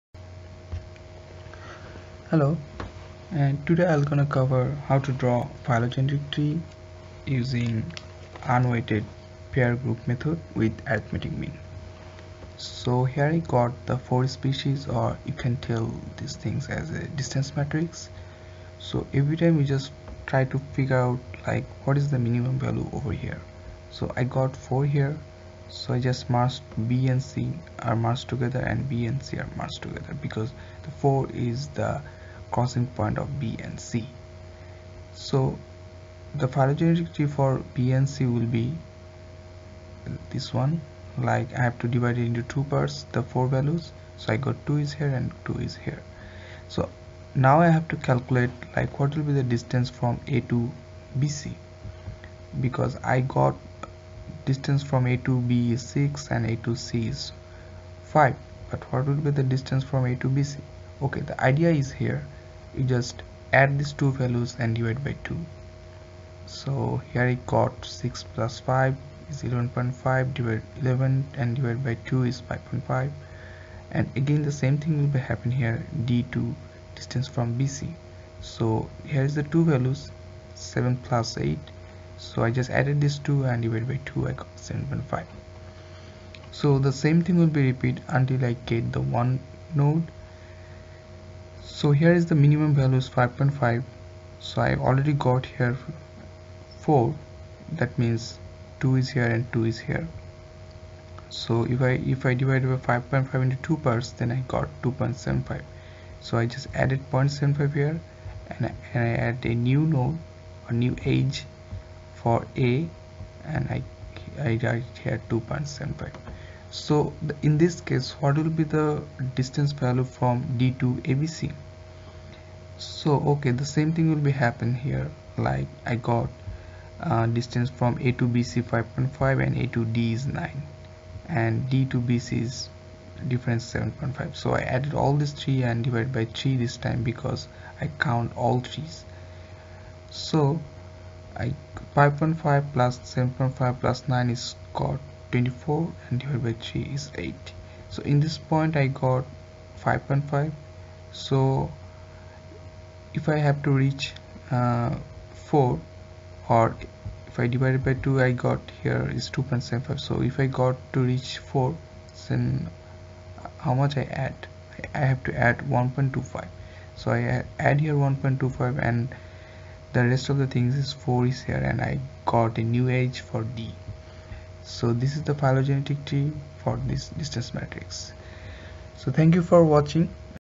How to draw phylogenetic trees UPGMA Method
Phylogenetic Trees generally dealing with missing information